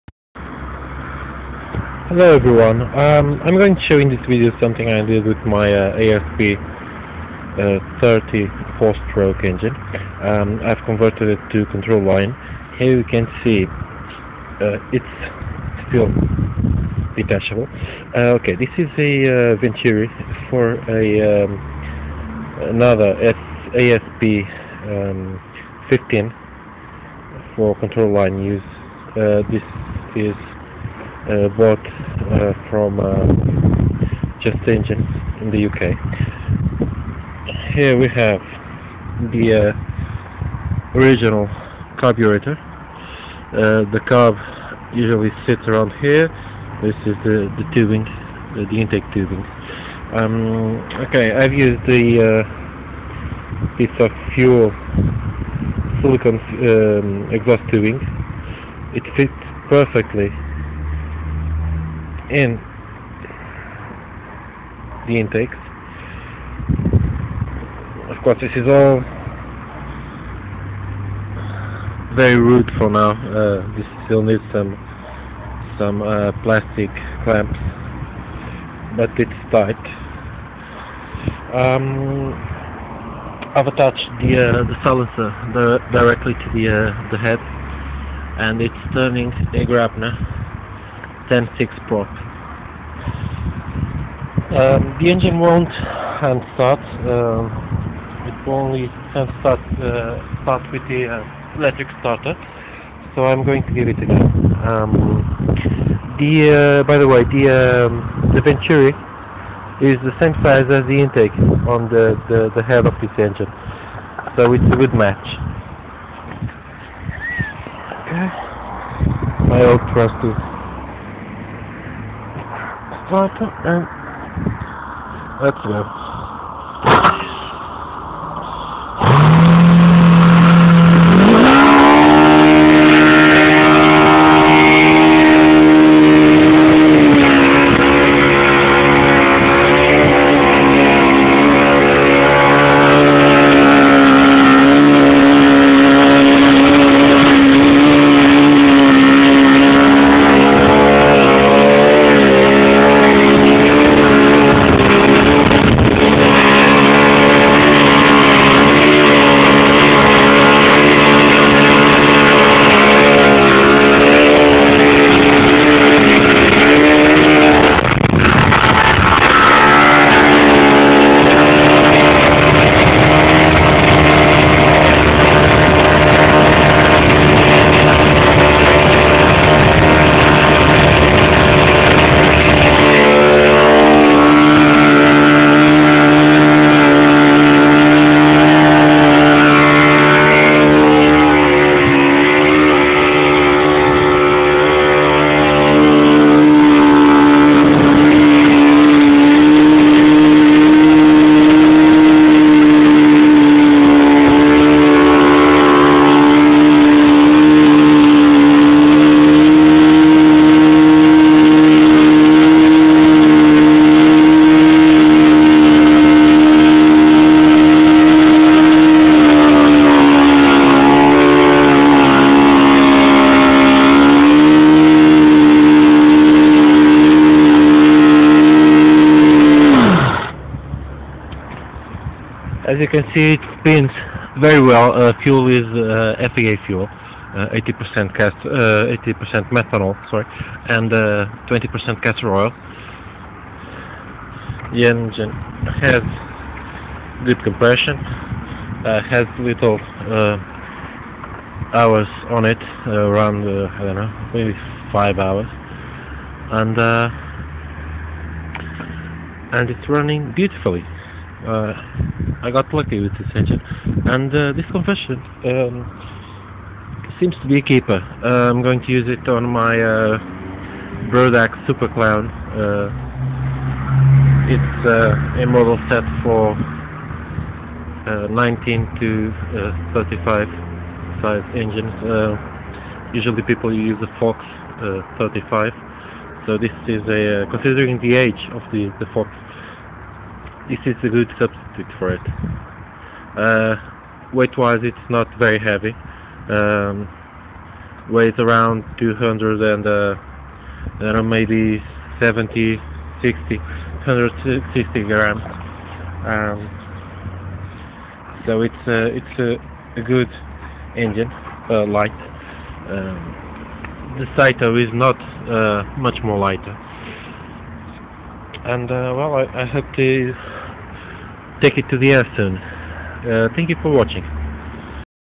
ASP FS-30AR control line conversion
This is a rough conversion I've made today on my trusty ASP FS30 to be used as a control-line engine.
I know, I could've just stuck the throttle arm open, but that's the easy way out.

As I had an extra venturi from my ASP 15 C/L, after some measurements I decided to cut the intake manifold 3mm above the carby and fit the ventury with a silicone tubing as used as exhaust diverters.

The intake diameter at the head matches perfectly the venturi, as both have the same size, 4mm.

Max rpm recorded was 9400rpm. Fuel is FAI fuel, 20% castor oil/ 80% methanol and the prop is a Graupner 10x6.
The engine seems to run better right now at 8900rpm on the bench, plenty of power for a stunt model designed for a Fox 35.